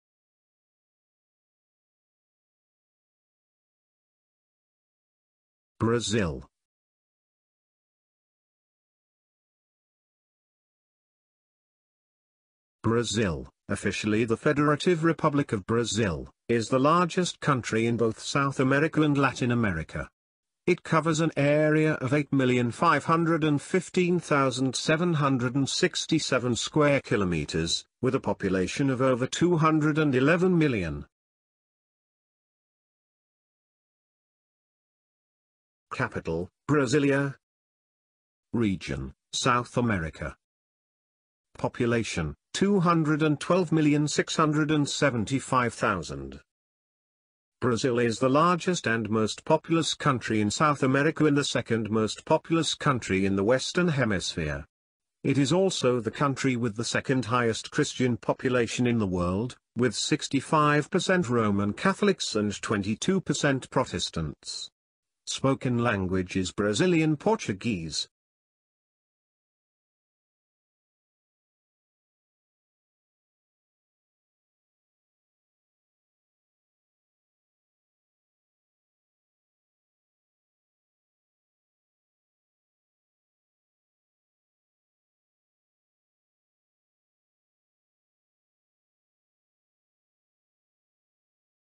Capital, region, flag, population and other details of Brazil (#6 World’s Most Populous Country)
Capital, region, flag, population and other details of Brazil (#6 World’s Most Populous Country) with Audio Description

Brazil
Brazil, officially the Federative Republic of Brazil, is the largest country in both South America and Latin America. It covers an area of 8,515,767 square kilometres, with a population of over 211 million.

Capital: Brasilia
Region: South America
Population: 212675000
Brazil is the largest and most populous country in South America and the second-most populous country in the Western Hemisphere. It is also the country with the second-highest Christian population in the world, with 65% Roman Catholics and 22% Protestants. Spoken language is Brazilian Portuguese.

Source: Population clock and IBGE